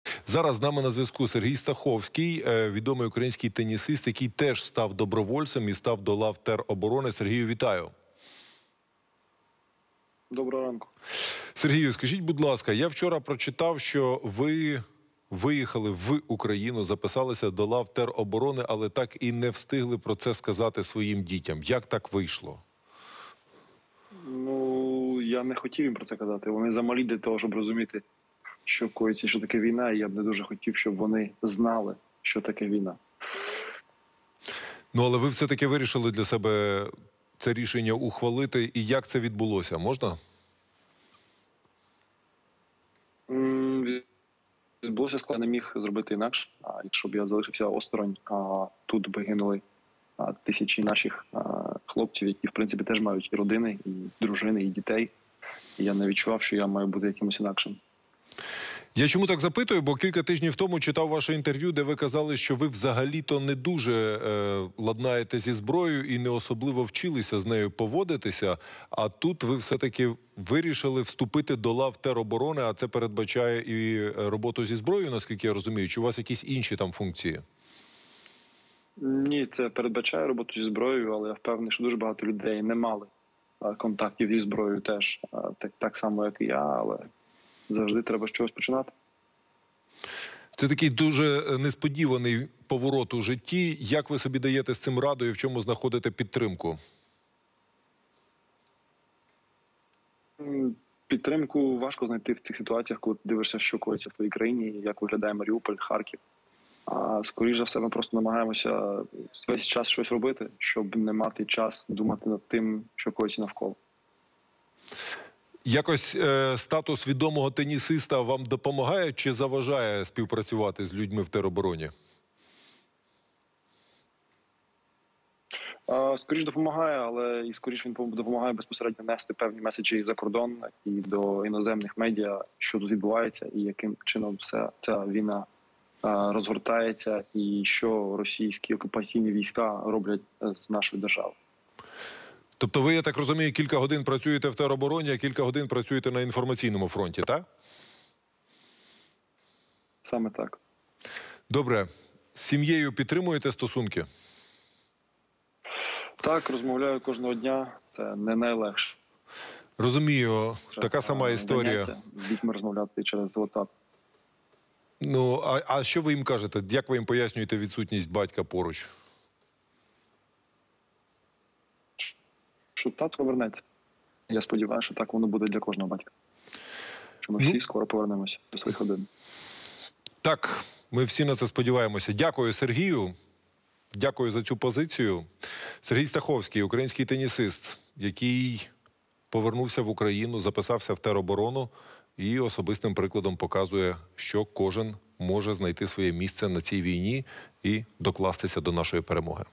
Ракетку на рушницю: український тенісист Сергій Стаховський став добровольцем
Сергій Стаховський про вступ до лав тероборони та боротьбу на інформаційному фронті.